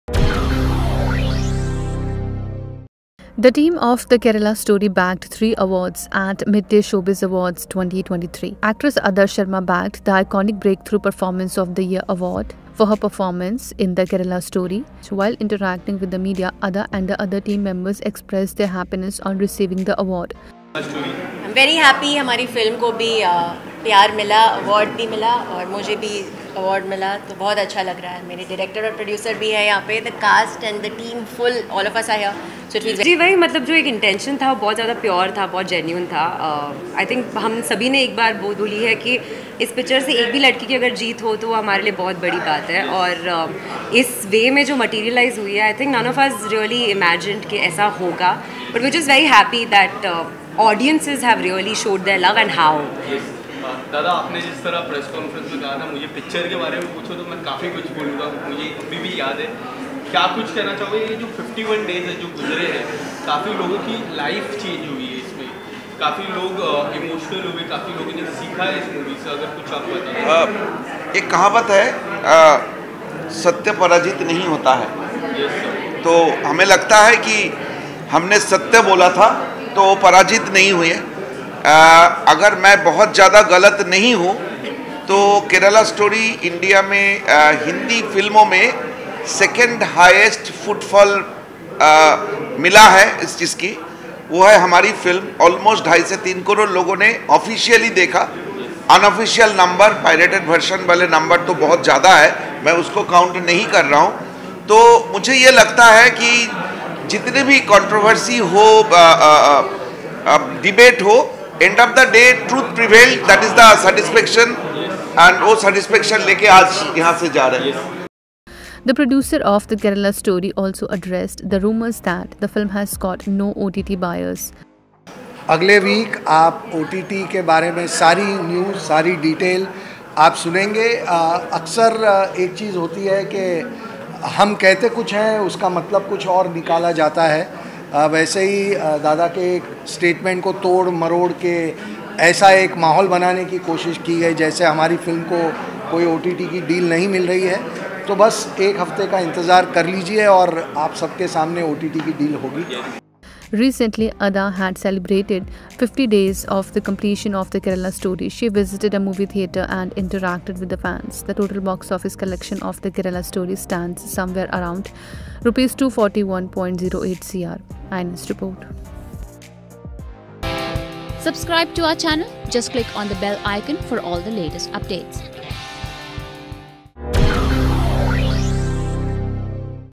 Adah Sharma bags award for her performance in 'The Kerala Story'
The team of 'The Kerala Story' bagged three awards at Midday Showbiz Awards 2023. During their interaction with the media, the team expressed their happiness on receiving the awards.